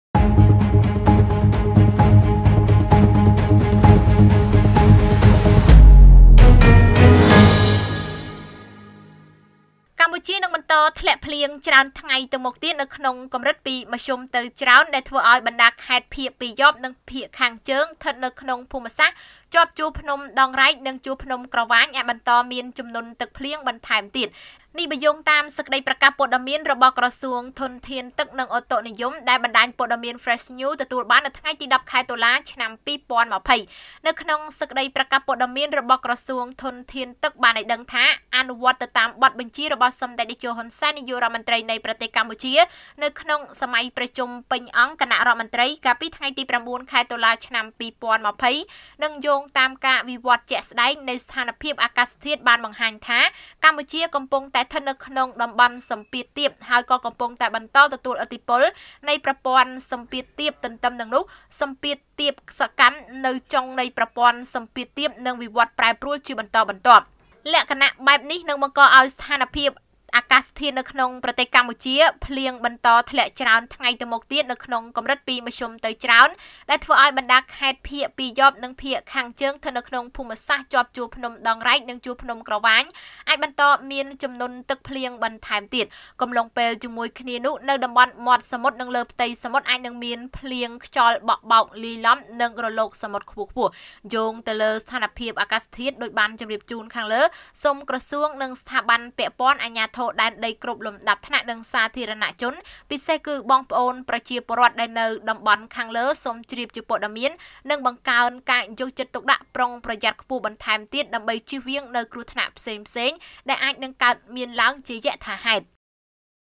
ក្រសួងធនធានទឹកប្រកាសទៀតថា កម្ពុជាបន្ដធ្លាក់ភ្លៀងច្រើនថ្ងៃទៅមុខទៀត, ក្រសួងអំពាវនាវឲ្យប្រជាពលរដ្ឋ...
ក្រសួងធនធានទឹកប្រកាសទៀតថា កម្ពុជាបន្ដធ្លាក់ភ្លៀងច្រើនថ្ងៃទៅមុខទៀត, ក្រសួងអំពាវនាវឲ្យប្រជាពលរដ្ឋ បង្កើនការយកចិត្តទុកដាក់ និងប្រុងប្រយ័ត្នខ្ពស់
 * រាយការណ៍ព័ត៌មានដោយកញ្ញា ឆេង សូរិយា, ទូរទស្សន៍ព័ត៌មាន Fresh News